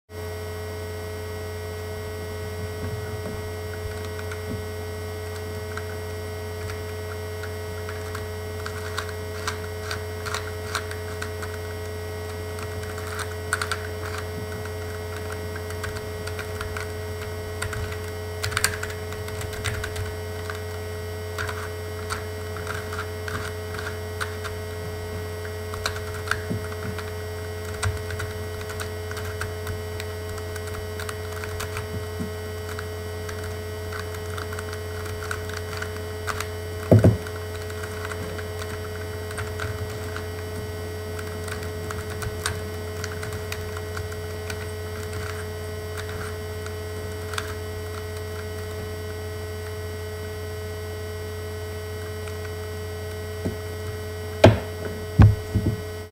Mini Dino Cube
This is my version of the dino cube. It is slightly smaller than a regular dino cube, as it measures 50mm to an edge instead of 57mm. I designed this using Rhino for Mac, had masters printed in Shapeways' transparent detail, and casted the pieces with Smooth-Cast 300. The turning is excellent, but the cube occasionally locks up.